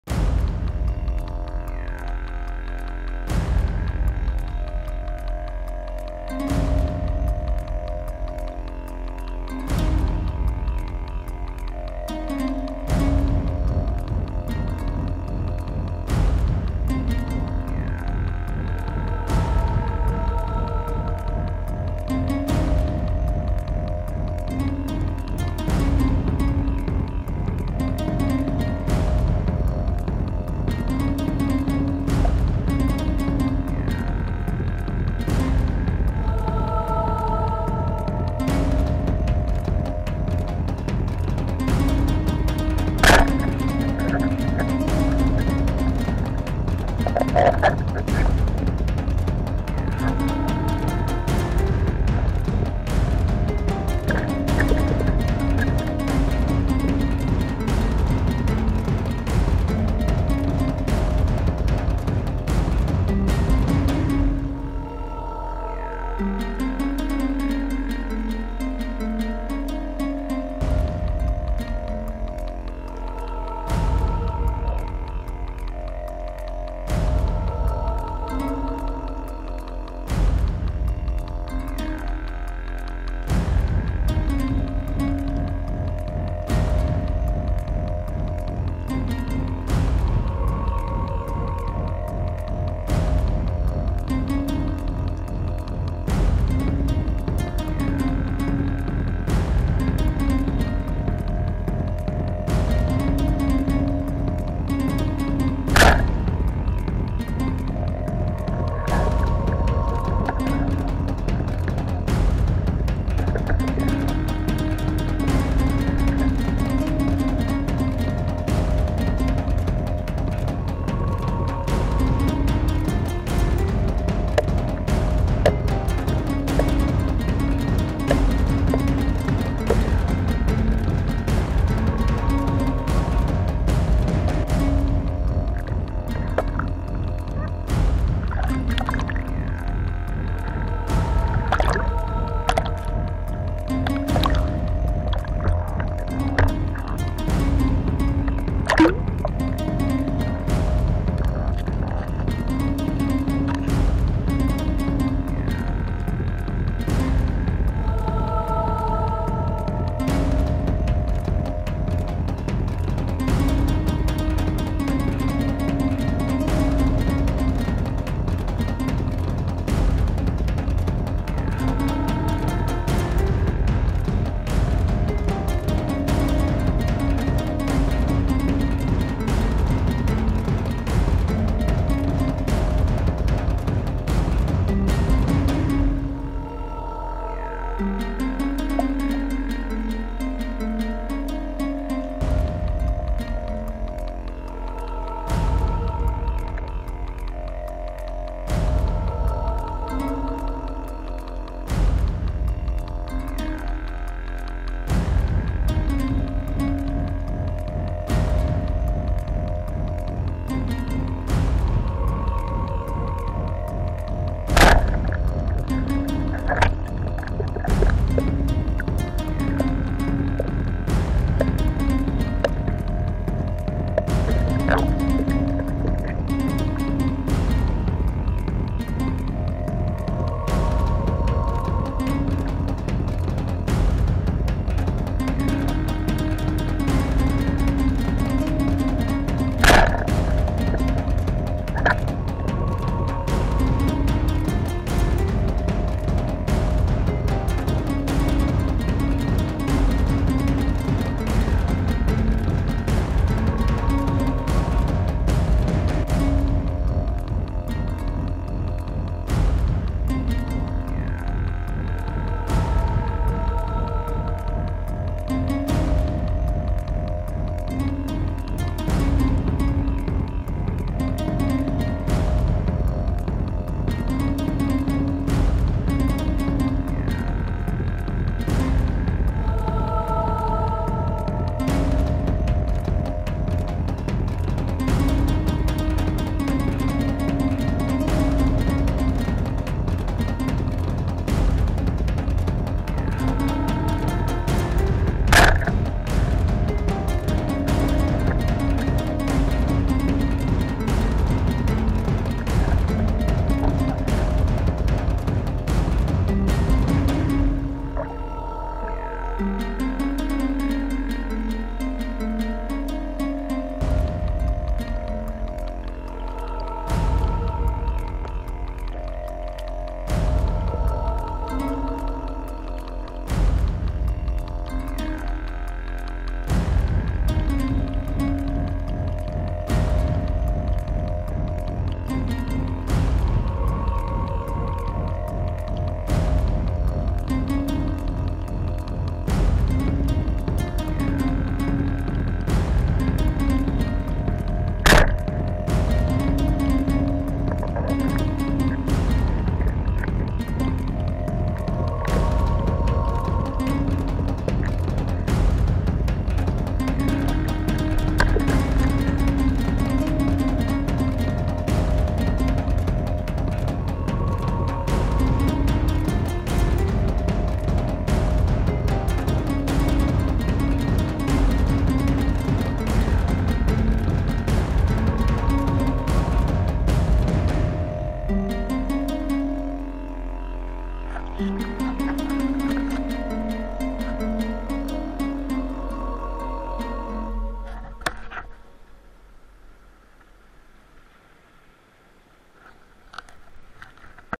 A PESCA IN UNO SPOT SEGRETO!!
OGGI MI ALLARGO UN Pò, IN UNA ZONA MOLTO BATTUTA IN ESTATE..IN CIRCA 13-16MT DI FONDO,MA FUORI STAGIONE REGALA QUALCHE BEL PESCE DAI MILLE COLORI... BUONA VISIONE.